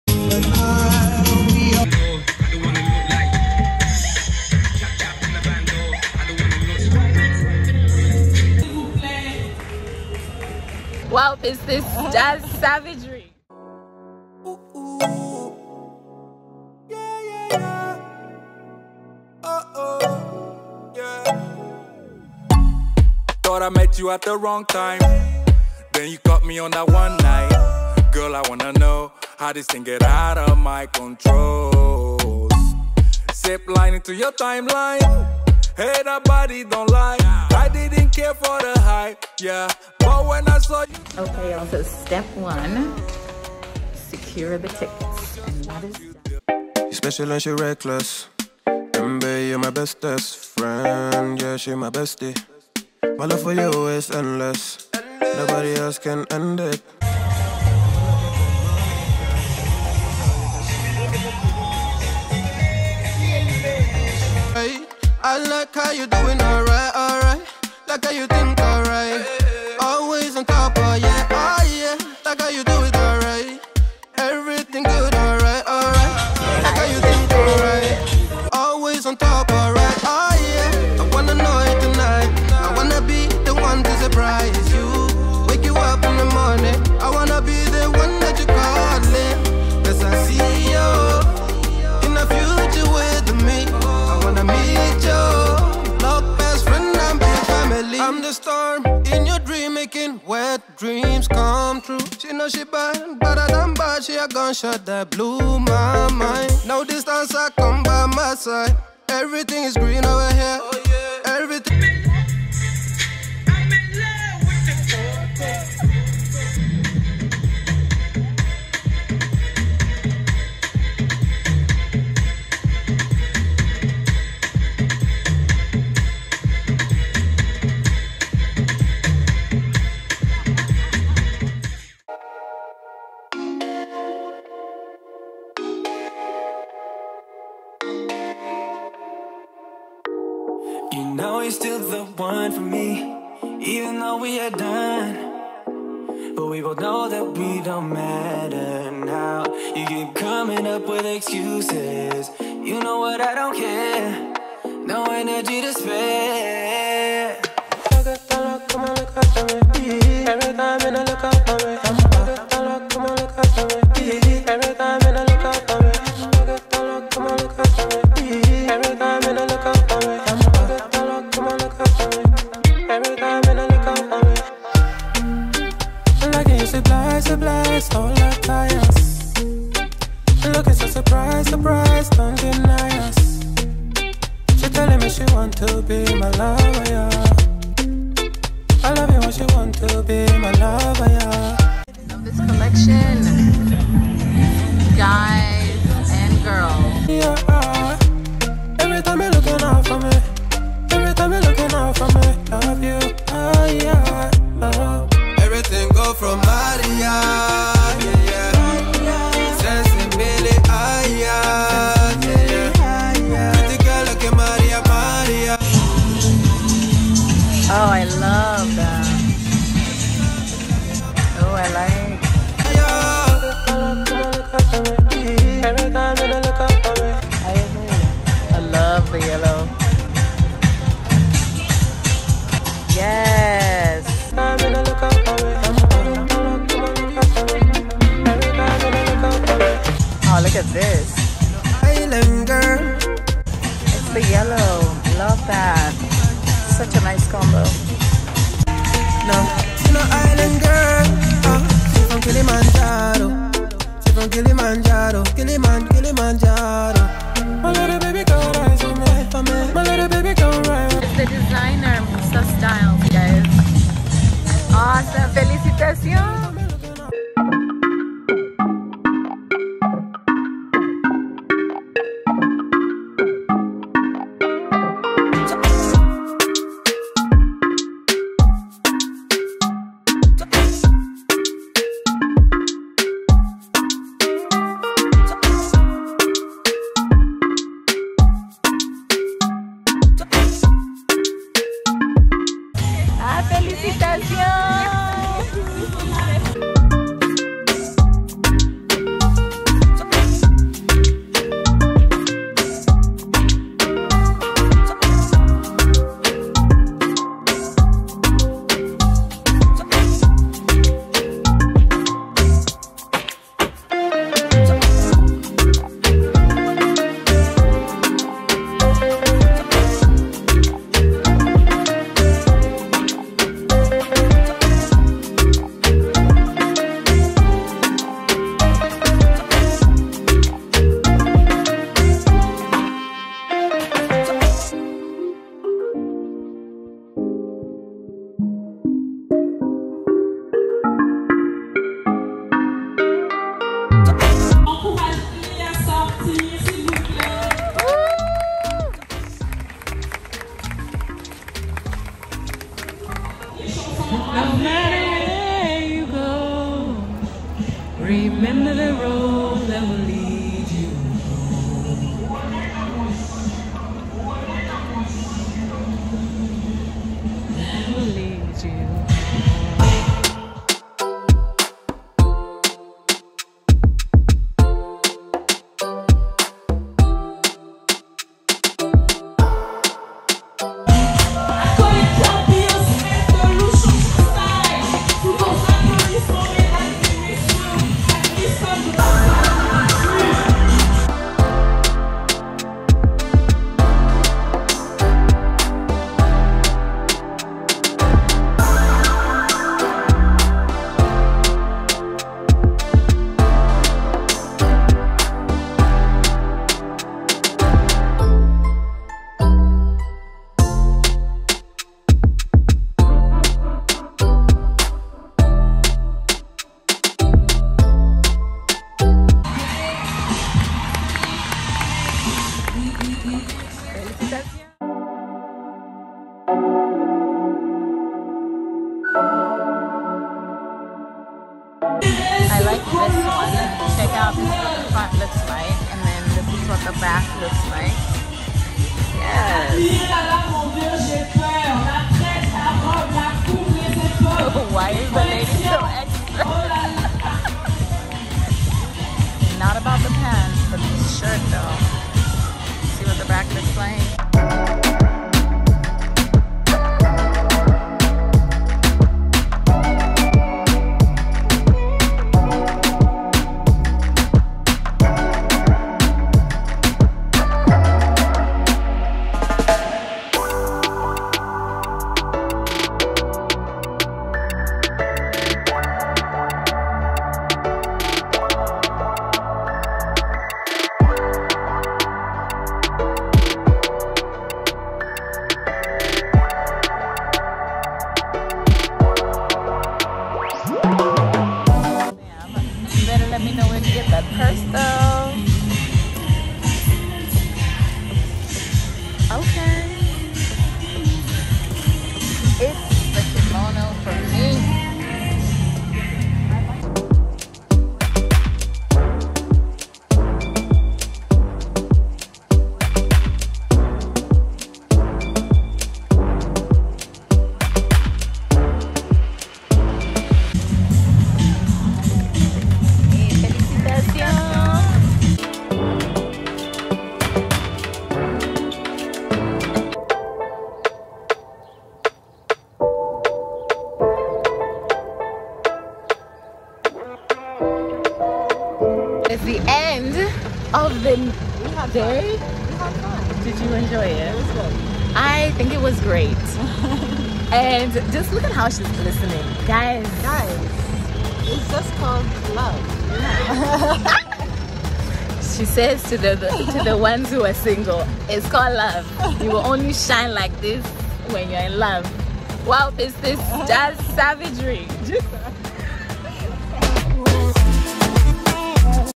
KINSHASA VLOG: DRC FASHION WEEK | COCKTAILS, FASHION SHOWS & GOOD VIBES | AFRICANCOCKTAIL
Hola mes amours! Today's vlog is one of my favorites because I get to share with you some images of the DRC that the media doesn't usually show. I get the privilege of sharing the work of some of my favorite creators! DRC Fashion Week showcases so much of the Congolese fashion sphere and I'm so glad that I can share it all with you!